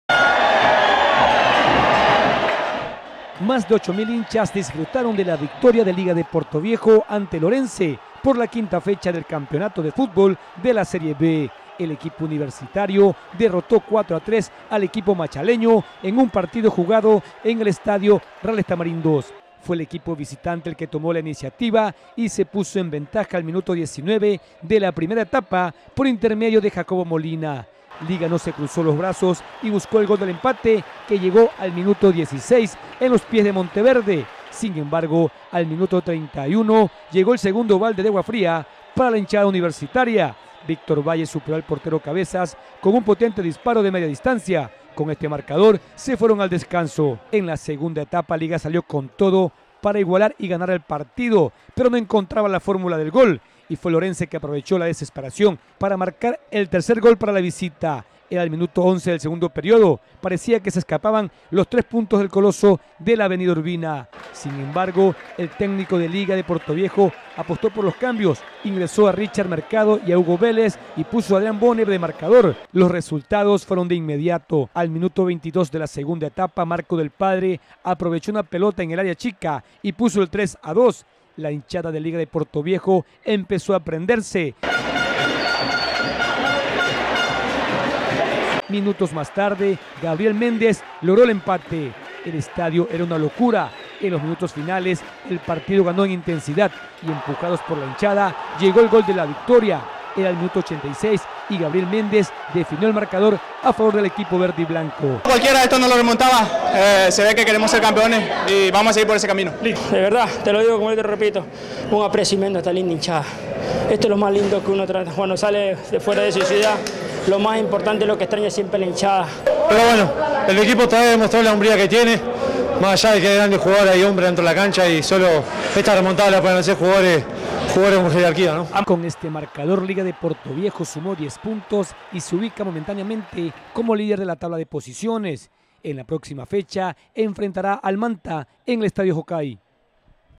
Liga de Portoviejo gana 4-3 y es puntero en la tabla de posiciones
Liga de Portoviejo remonta el marcador y gana 4-3 frente al Orense SC ⚽️